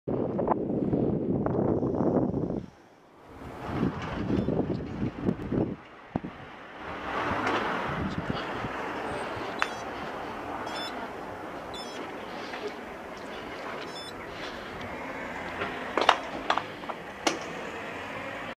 OJ2357 1932 Alvis Speed 20
Don't think I'll top this regards age of car spotted on my travels unless I get a Model T.
If you're interested in buying one you best sit down, prices are around £100k